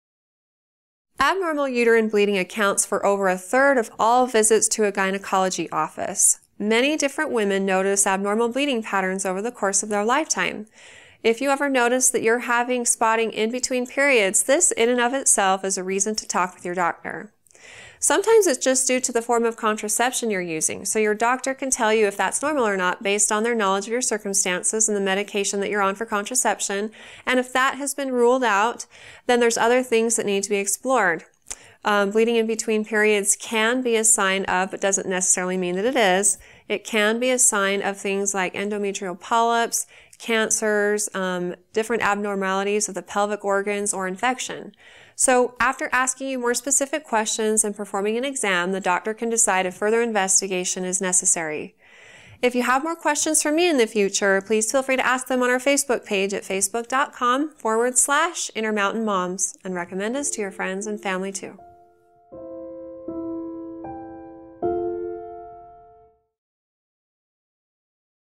Why am I spotting if I'm not on my period?
Abnormal uterine bleeding accounts for over a third of all visits to a gynecology office. Many different women notice abnormal bleeding patterns over the course of their lifetime. If you ever notice that you're having spotting in between periods, this, in and of itself, is a reason to talk with your doctor. Sometimes it's just due to the form of contraception you're using. So your doctor can tell you if that's normal or not, based on their knowledge of your circumstances and the medication that you're on for contraception. And if that has been ruled out, then there's other things that need to be explored. Bleeding in between periods can be a sign of - but doesn't necessarily mean that it is - it can be a sign of things like endometrial polyps, cancers, different abnormalities of the pelvic organs, or infection. So after asking you more specific questions and performing an exam, the doctor can decide if further investigation is necessary.